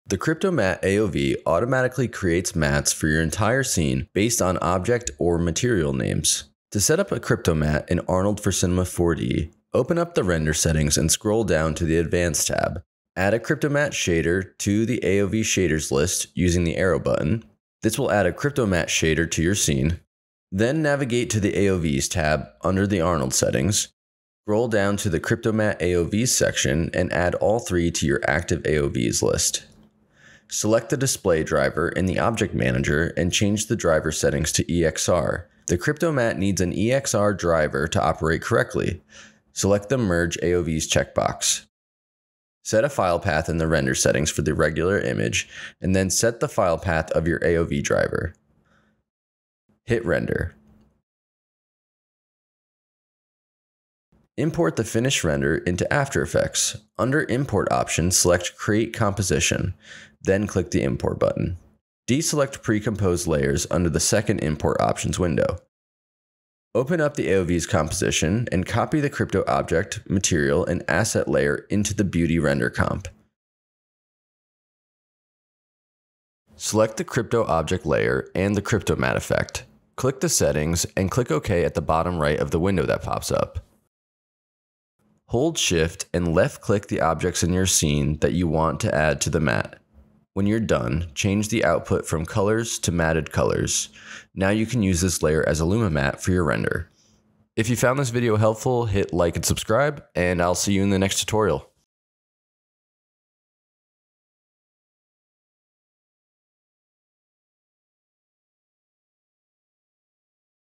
How to use Cryptomattes in Arnold + C4D
The cryptomatte AOV automatically creates mattes for your entire scene based on object or material names.

To setup a cryptomatte in Arnold for Cinema4D…

Open up the render settings and scroll down to the “Advanced” tab. Add a cryptomatte shader to the AOV shaders list using the arrow button.

This will add a cryptomatte shader to your scene.

Then navigate to the AOVs tab under the Arnold settings. Scroll down to the cryptomatte AOVs section and add all three to your active AOVs list.

Select the display driver in the object manager and change the driver settings to EXR. The cryptomatte needs an EXR driver to operate correctly. Select the Merge AOVs checkbox.

Set a file path in the render settings for the regular image and then set the file path of your AOV driver.

Hit render.

Import the finished render into after effects. Under “Import Options” select create composition. Then click the import button.

De-select pre-compose layers under the second “Import Options” window.

Open up the AOVs composition and copy the crypto object, material and asset layer into the beauty render comp.

Select the crypto object layer and the Cryptomatte effect. Click the settings and Click “OK” at the bottom right of the window that pops up.

Hold shift and left click the objects in your scene that you want to add to the matte. When you’re done change the output from Colors to Matted Colors. Now you can use this layer as a luma matte for your render.